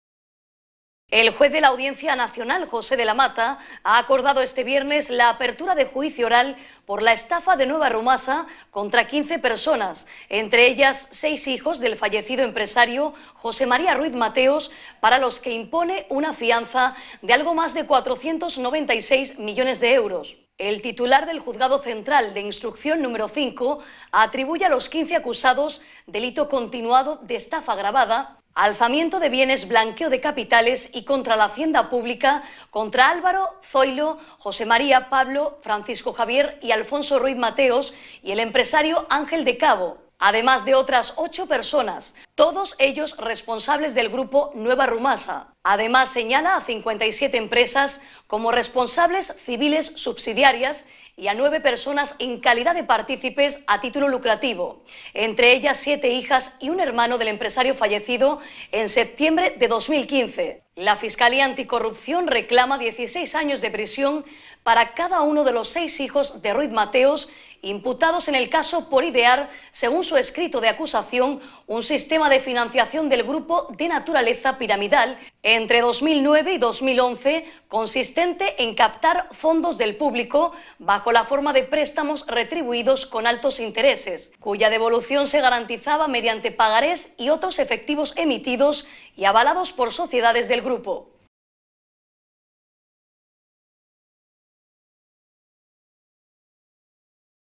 Abren juicio oral Nueva Rumasa e impone fianza 500 millones 6 hijos Ruiz-Mateos - Málaga 24h TV
El juez de la Audiencia Nacional José de la Mata ha acordado este viernes la apertura de juicio oral por la estafa de Nueva Rumasa contra 15 personas, entre ellas seis hijos del fallecido empresario José María Ruiz-Mateos, para los que impone una fianza de algo más de 496 millones de euros.
El titular del Juzgado Central de Instrucción número 5 atribuye a los 15 acusados delito continuado de estafa agravada, alzamiento de bienes, blanqueo de capitales y contra la Hacienda pública contra Álvaro, Zoilo, José María, Pablo, Francisco Javier y Alfonso Ruiz-Mateos y el empresario Ángel de Cabo, además de otras ocho personas, todos ellos responsables del Grupo Nueva Rumasa. Además, señala a 57 empresas como responsables civiles subsidiarias y a nueve personas en calidad de partícipes a título lucrativo, entre ellas siete hijas y un hermano del empresario fallecido en septiembre de 2015. La Fiscalía Anticorrupción reclama 16 años de prisión para cada uno de los seis hijos de Ruiz-Mateos imputados en el caso por idear, según su escrito de acusación, un sistema de financiación del Grupo de naturaleza piramidal entre 2009 y 2011 consistente en captar fondos del público bajo la forma de préstamos retribuidos con altos intereses, cuya devolución se garantizaba mediante pagarés y otros efectos emitidos y avalados por sociedades del Grupo.

English:
The judge of the National Court Jose de la Mata has agreed on Friday to open an oral trial for the New Rumasa scam against 15 people, including six children of the deceased businessman Jose Maria Ruiz-Mateos, for which he imposes a bond on something More than 496 million euros.
The head of the Central Court of Instruction number 5 attributes to the 15 accused continued aggravated felony, uprising of assets, money laundering and against the public treasury against Alvaro, Zoilo, Jose Maria, Pablo, Francisco Javier and Alfonso Ruiz-Mateos and The businessman Ángel de Cabo, as well as eight other people, all of them responsible for the Nueva Rumasa Group. In addition, it points to 57 companies as responsible civilian subsidiaries and nine people as profit-takers, including seven daughters and one brother of the entrepreneur who died in September 2015. The Anti-Corruption Prosecution claims 16 years imprisonment for each of the six Sons of Ruiz-Mateos charged in the case for devising, according to his indictment, a system of financing the Group of pyramidal nature between 2009 and 2011 consisting of raising funds from the public in the form of high interest rate repayment loans, whose repayment Guaranteed through promissory notes and other effects issued and guaranteed by Group companies.